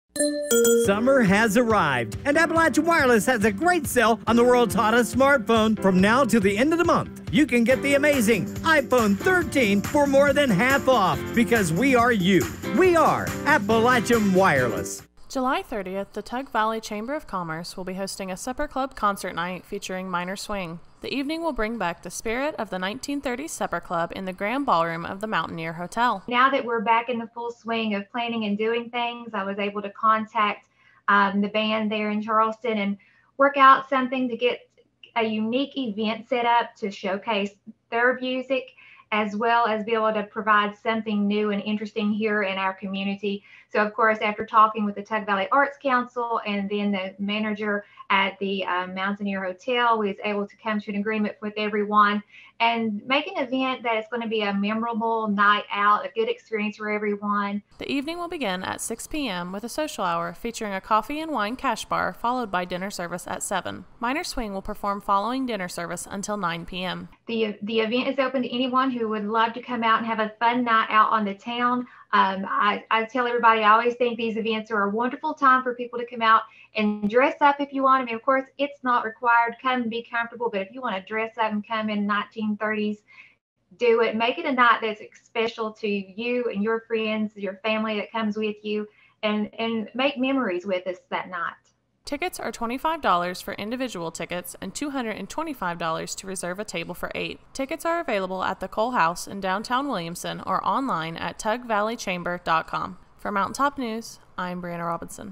Supper Concert to Relive the 1930s
The Tug Valley Chamber of Commerce will host a supper concert on July 30th that will bring back the spirit of the 1930s.